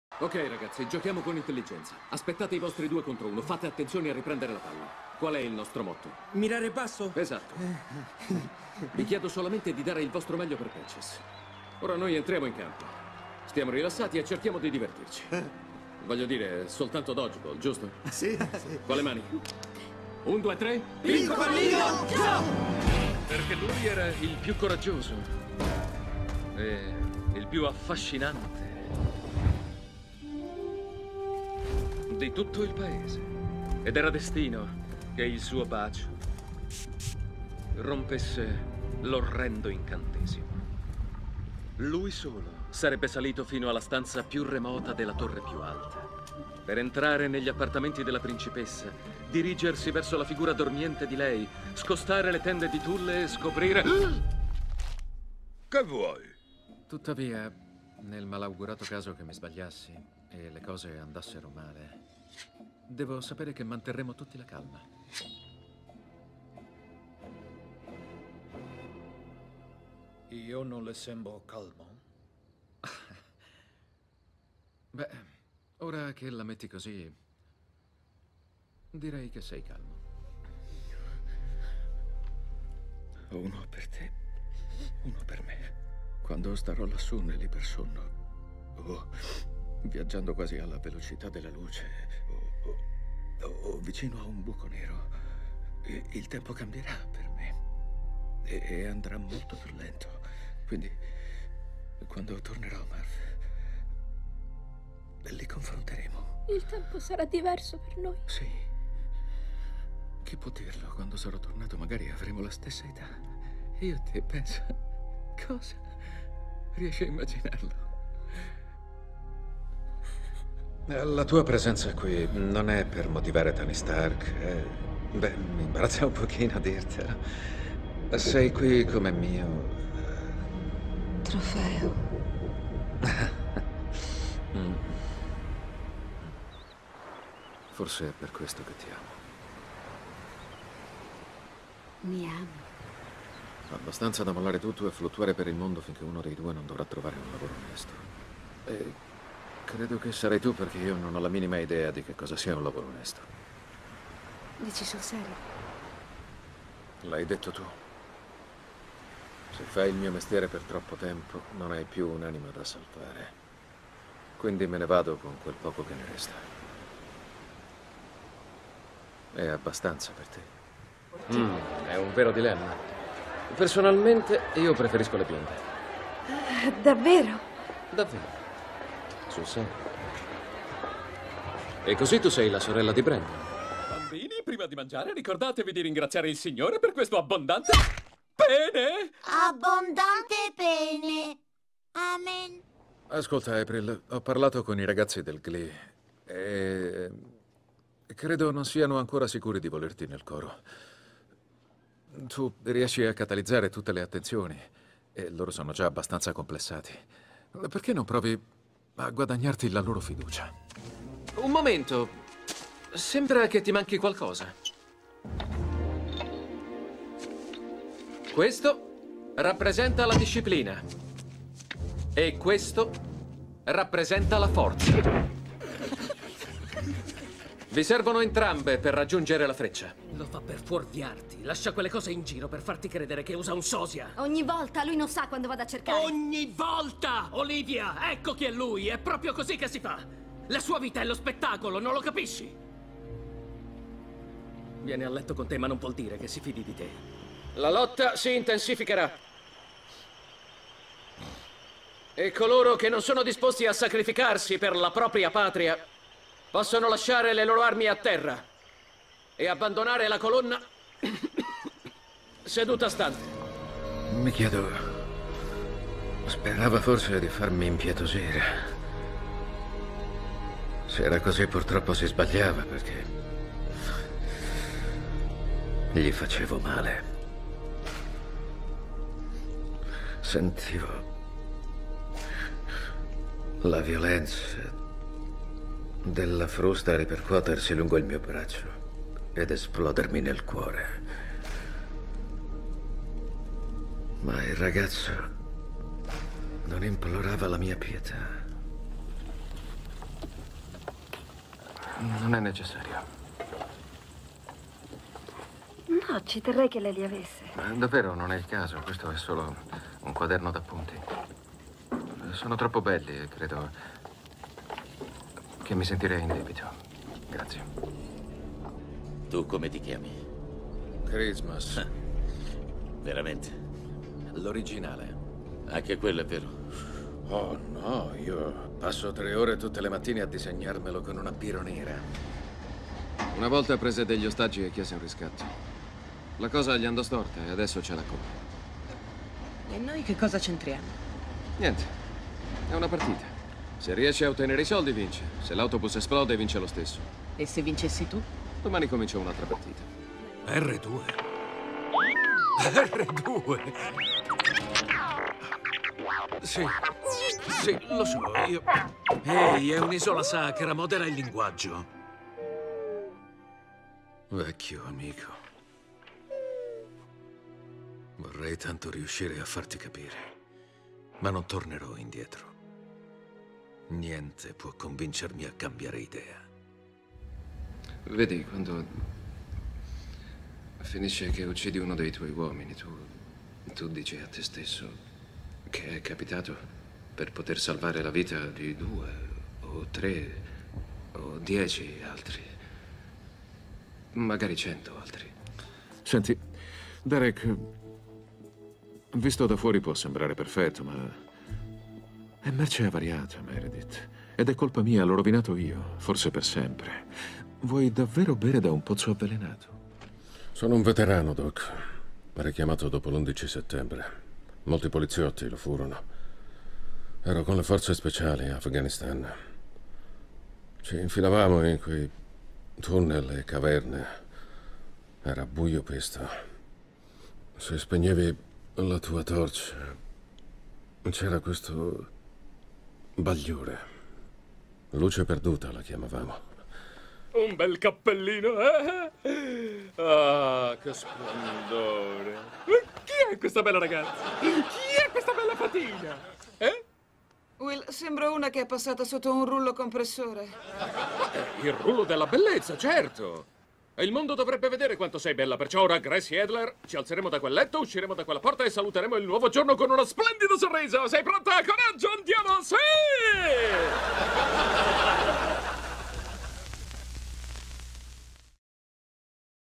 Francesco Prando - Il mondo del doppiaggio [KTS]
Dalla voce di 007 (Daniel Craig) a quella di Ned Flanders passando per Dylan di Beverly Hills 90210. Dal "cattivo" di Iron Man 3 (Guy Pearce) al Principe Azzurro di Shrek. Da Matthew McConaughey (Interstellar) a Michael Fassbender (Bastardi senza gloria). La bellezza della propria voce e la sua maestria nell'usarla fanno di Francesco Prando uno dei più grandi doppiatori contemporanei italiani.